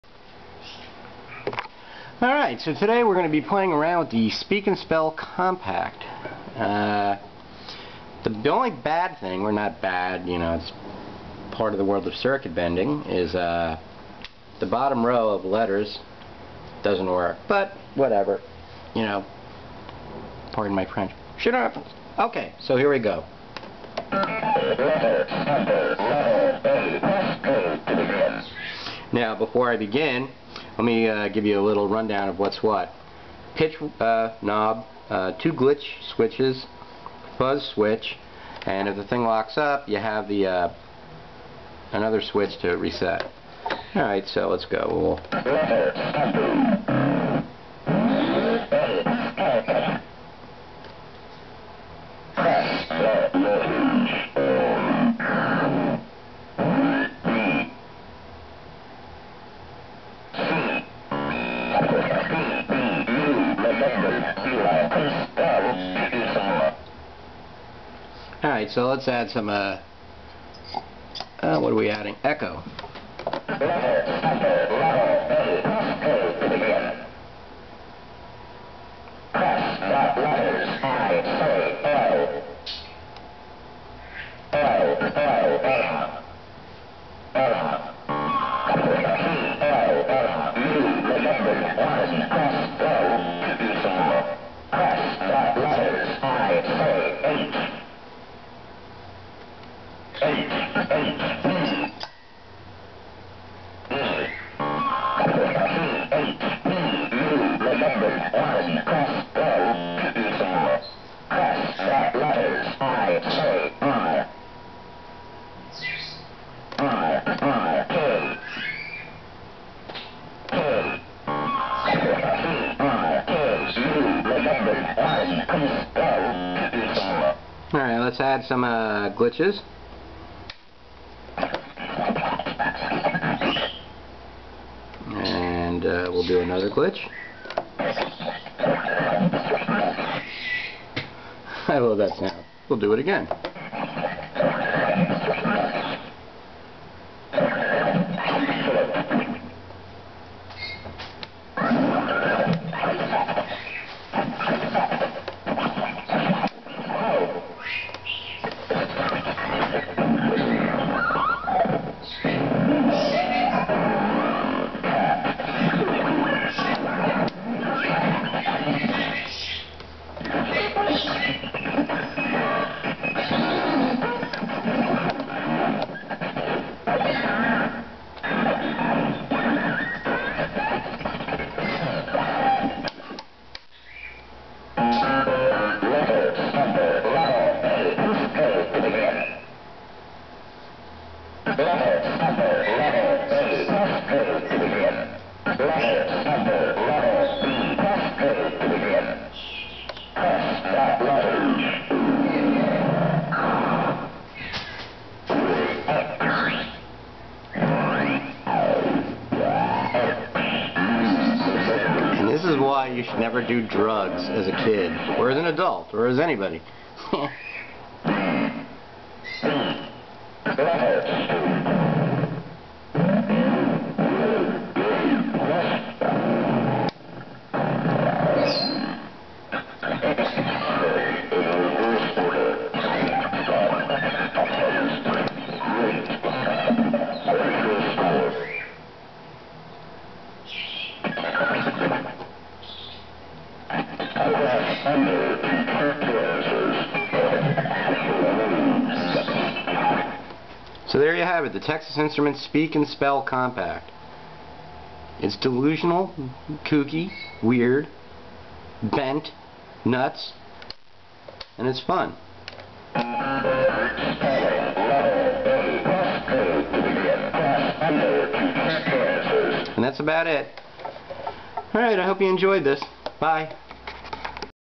Speak n Spell Compact edition.
A speak n spell compact edition with 2 glitch switches as well as "fuzz" switch.